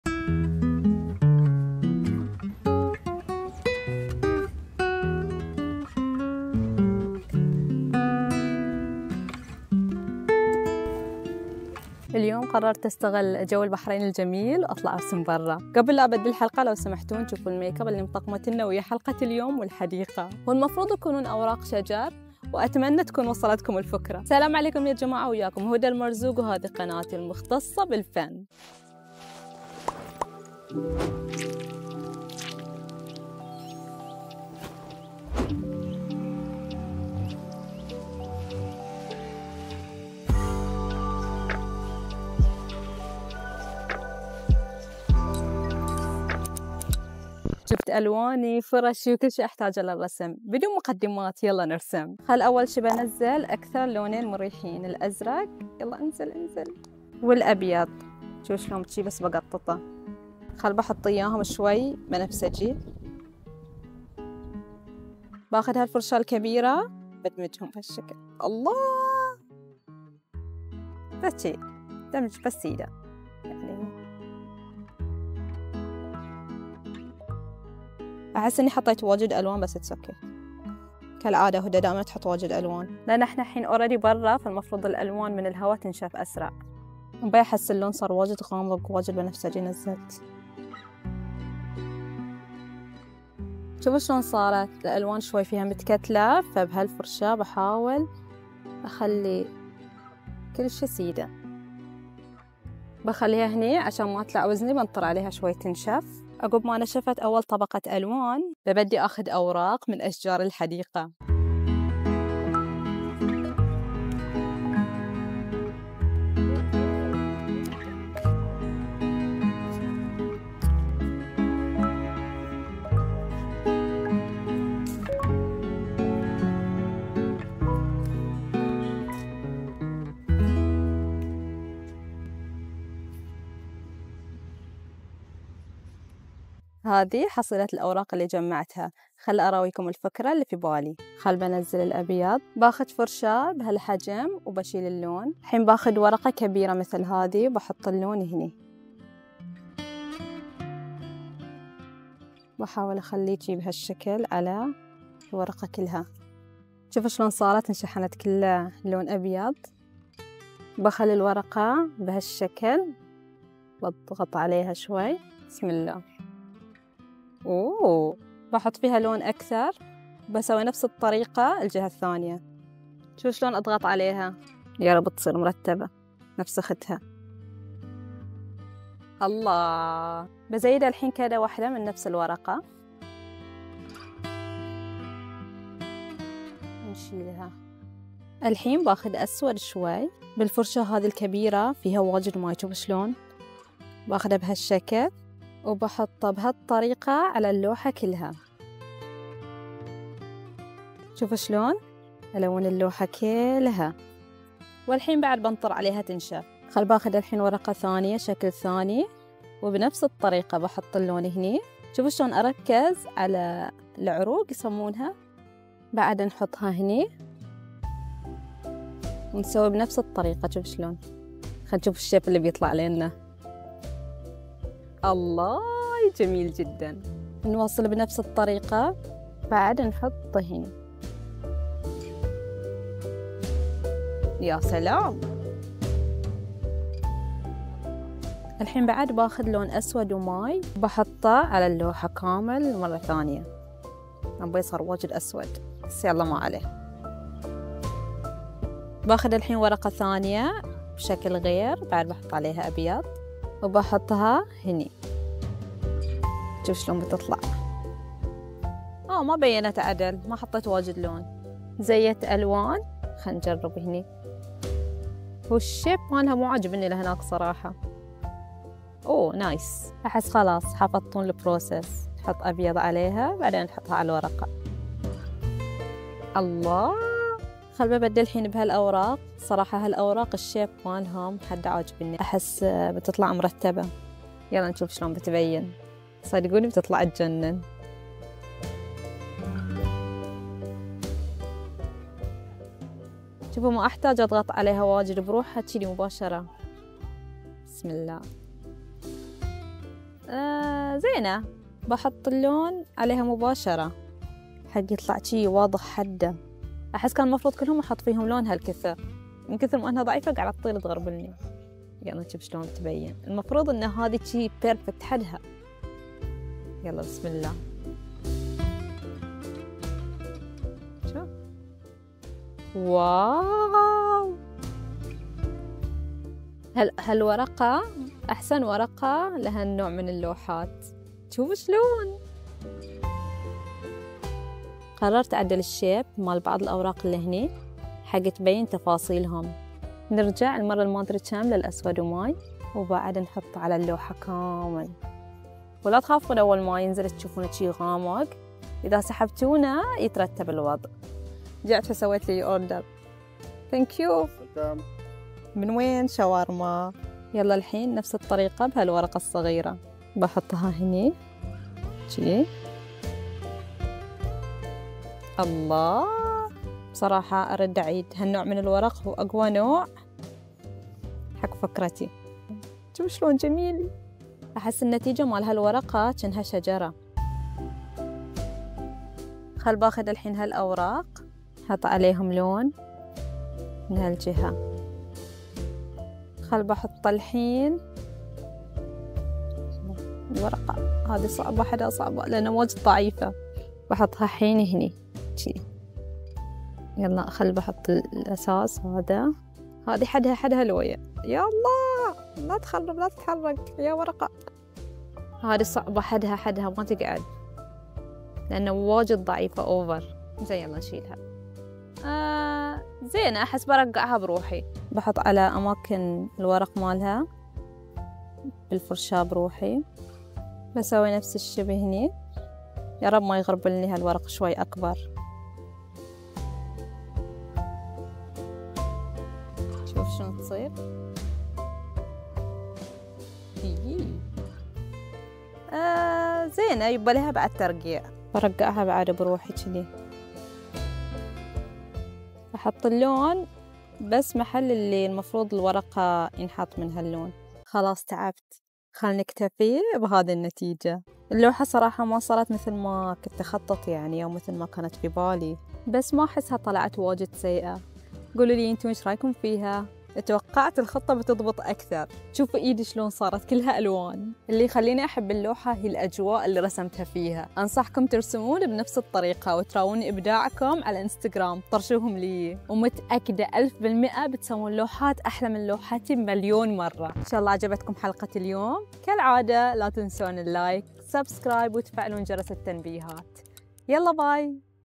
رسم في الحديقة, بأوراق الأشجار 🌿🎨
جربت أرسم بأوراق الأشجار, تجربة رسم لأول مرة و النتيجة مو مثل ماتوقعت 💔

ــــــــــــــــــــــــــــــــــــــــــــــــــــــــ
حساباتي في السوشل ميديا: